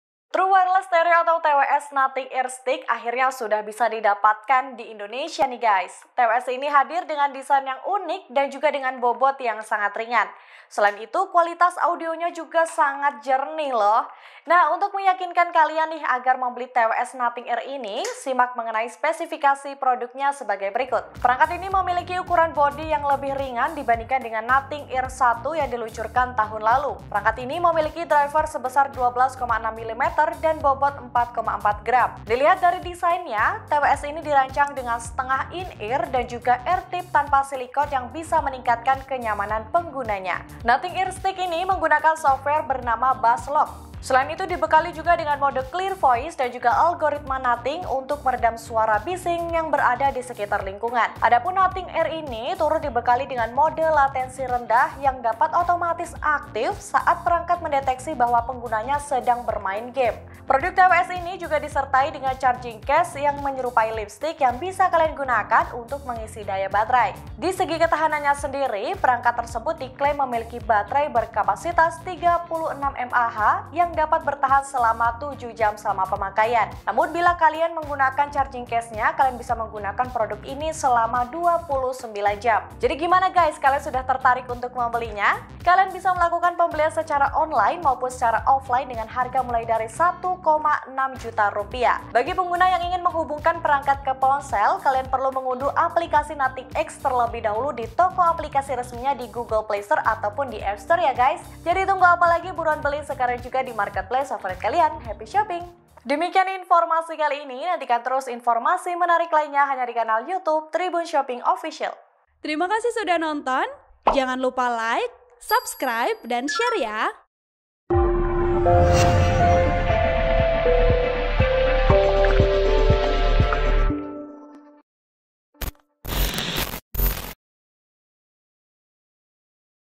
Kualitas Audio Super Jernih, Ini TWS Terbaru Nothing Ear Stick di Indonesia, Harga Rp 1 Jutaan
TRIBUN-SHOPPING.COM- True Wireless Stereo (TWS) Nothing Ear Stick akhirnya sudah bisa didapatkan di Indonesia nih guys.

TWS ini hadir dengan desain yang unik dan dengan bobot yang ringan.

Selain itu, kualitas audionya juga diklaim sangat jernih loh.

Nah untuk meyakinkan kalian agar membeli TWS Nothing Ear ini, simak mengenai spesifikasi produknya sebagai berikut.

Yuk dapatkan produknya disini.

TWS Nothing Ear (Stick)